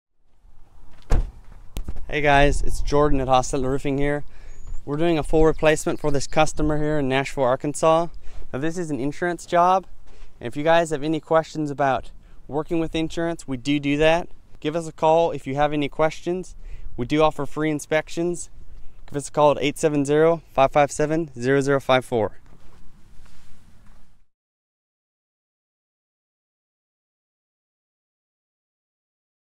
Nashville, AR. Roof Replacement
Here's Jordan showing us a residential roof replacement in Nashville,AR. We want to provide you with everything you need to make an informed decision on your next roofing project.